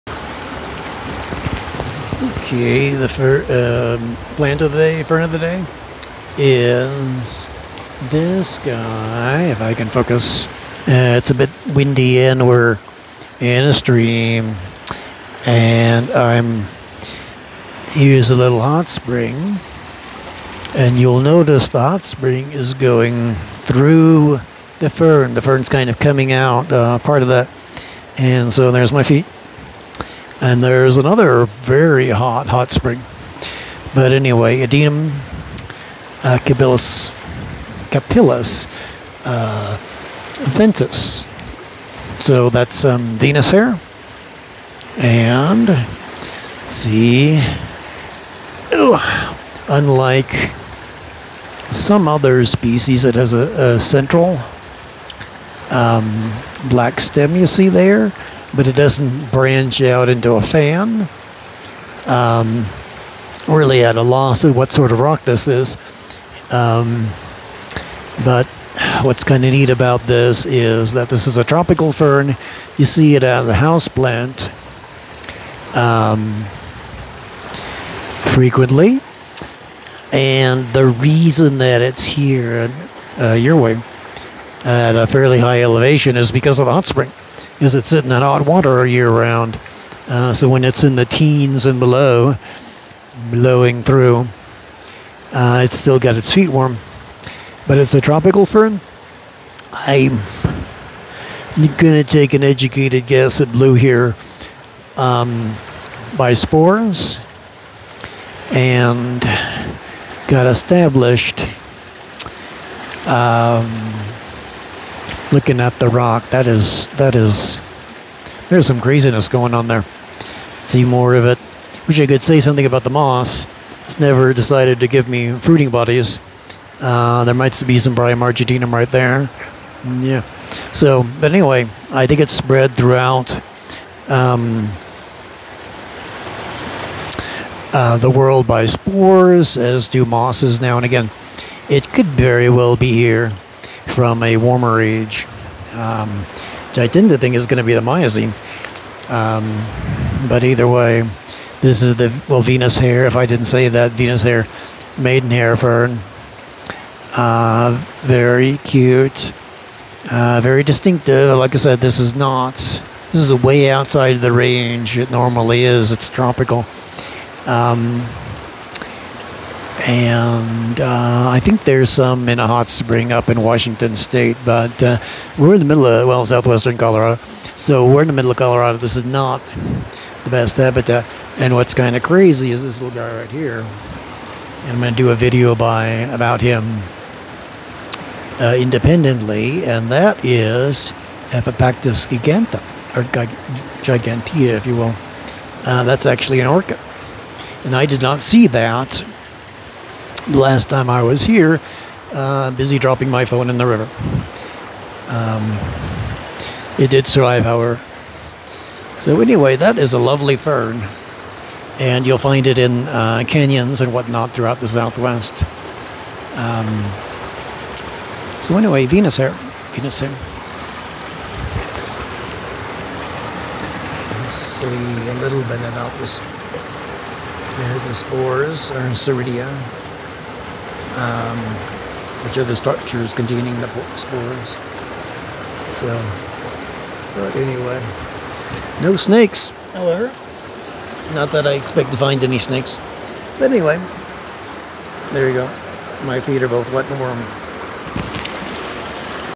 Venus hair fern, Adiantum capillus veneris, in a hot spring.
Ouray, Colorado, USA,
Ice park canyon.